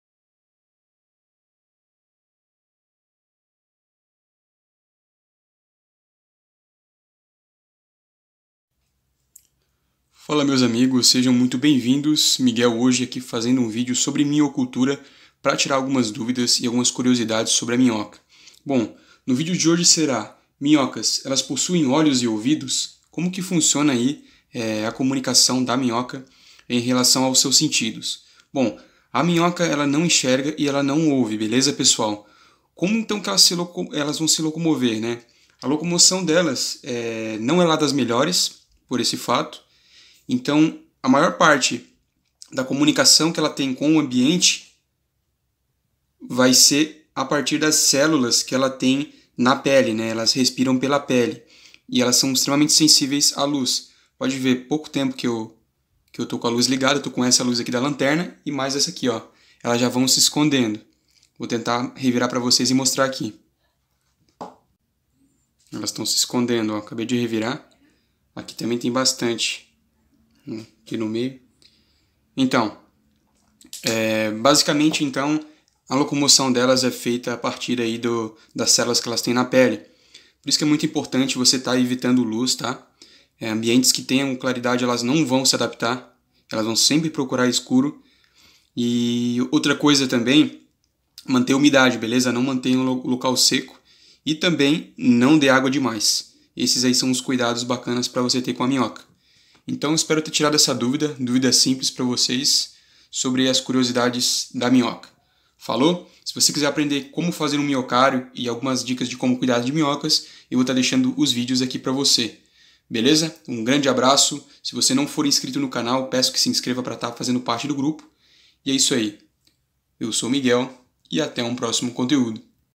AS MINHOCAS TEM OLHOS?
No vídeo falo sobre como a minhoca se locomove.
A minhoca possui sentidos diferentes dos quais imaginamos.
Dicas com minhocario
Minhocário caseiro
Como cuidar das minhocas
como criar minhocas